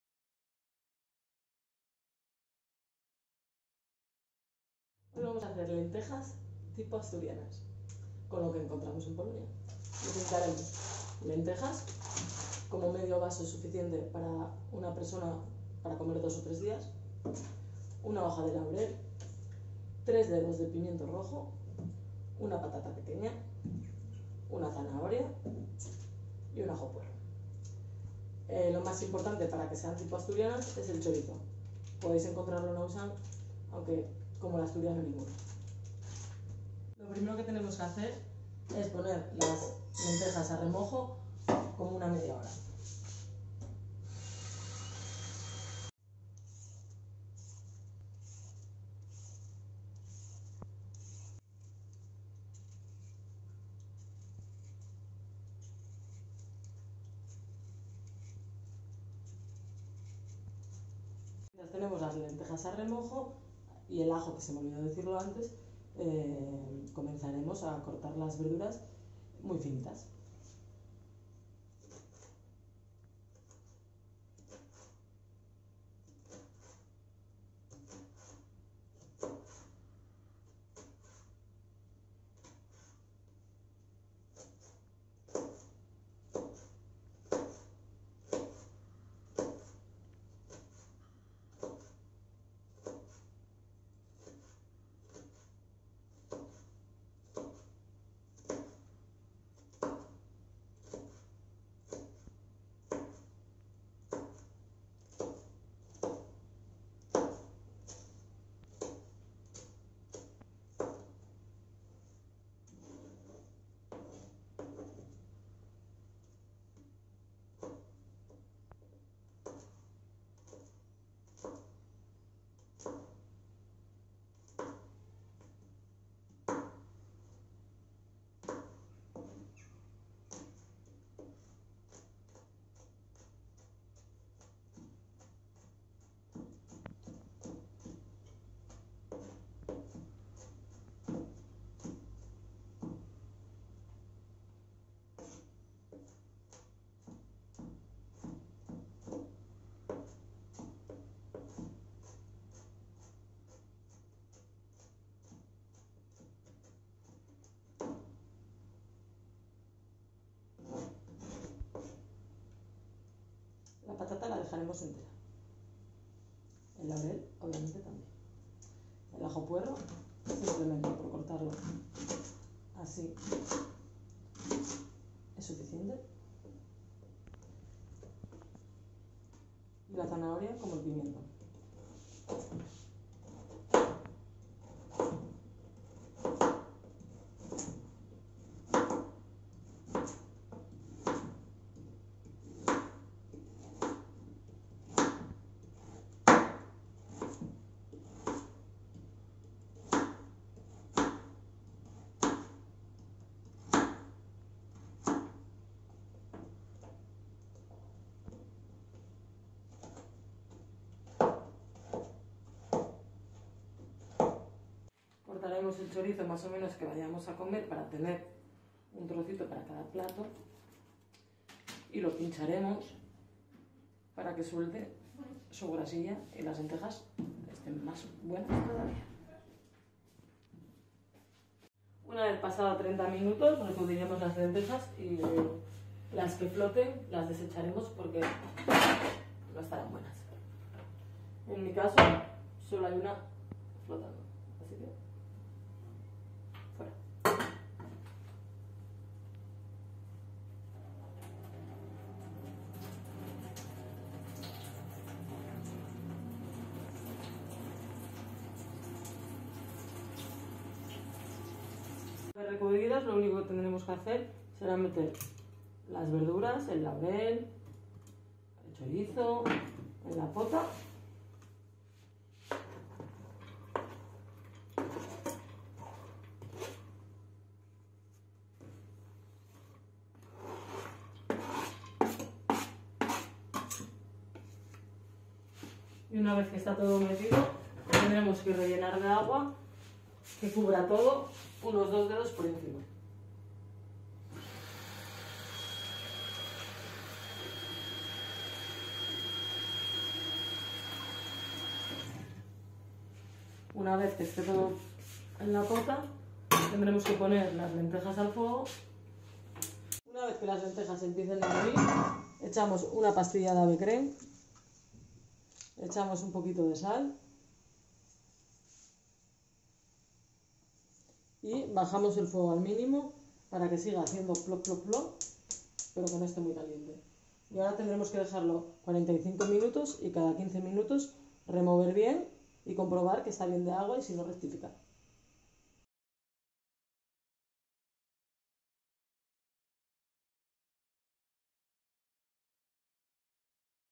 Lentejas z Hiszpanii. Zagraniczni wolontariusze gotują
Oto kolejna propozycja naszej wolontariuszki Carli.  Zapraszamy do Hiszpanii na tradycyjną  potrawę  LENTEJAS.

To typowe danie jednogarnkowe, które jest doskonałą alternatywą dla dobrze znanego w Polsce gulaszu.